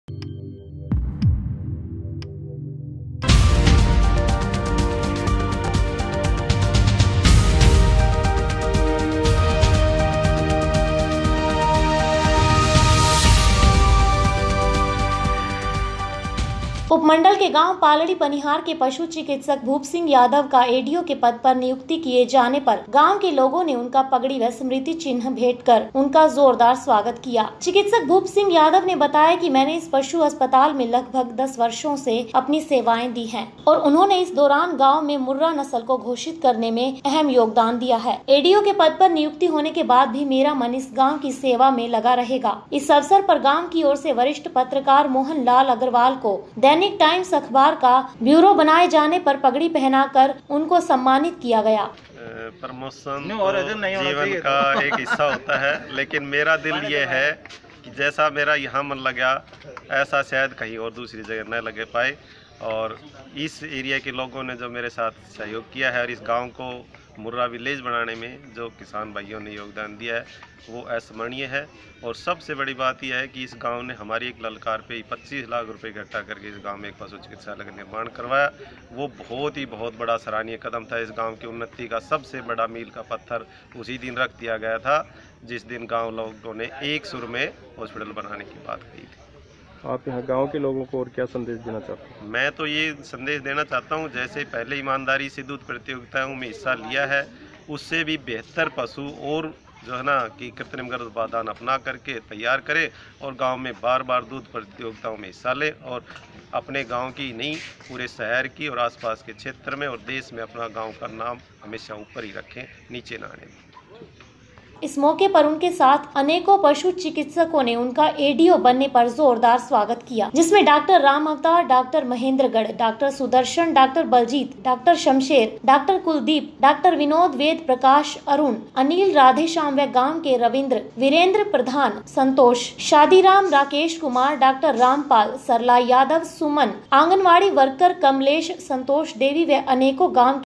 महेंद्रगढ़ में हुआ सम्मान समारोह ग्राम वासियों ने पगड़ी पहनाकर किया सम्मान
महेंद्रगढ़ से दिनेश यादव की रिपोर्ट हर न्यूज़ हरियाणा